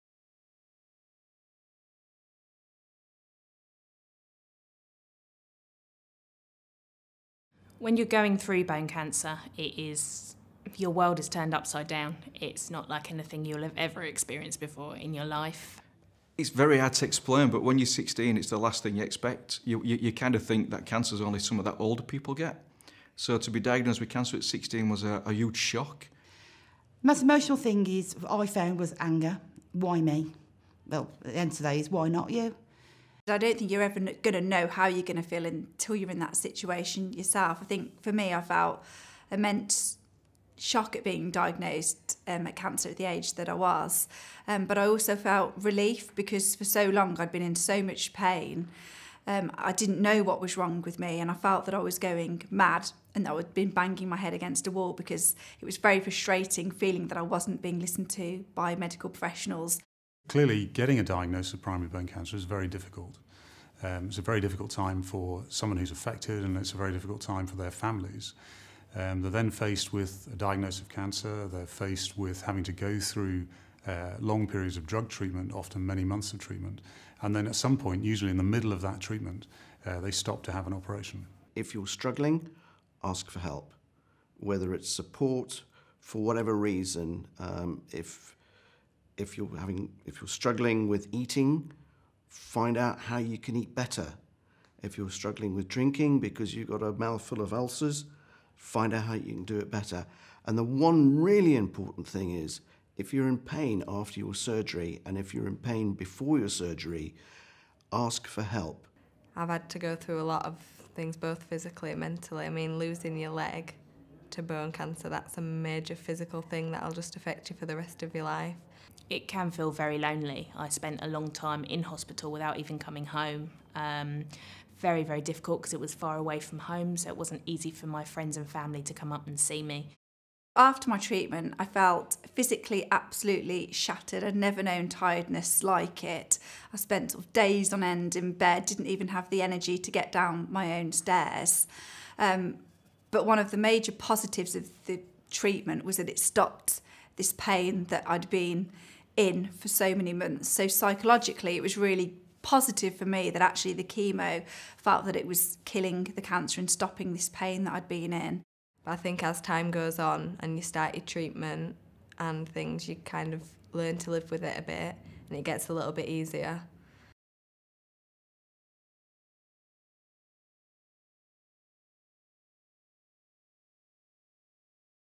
Primary bone cancer: how diagnosis and treatment affected me
A group of bone cancer patients share their experience of diagnosis and treatment.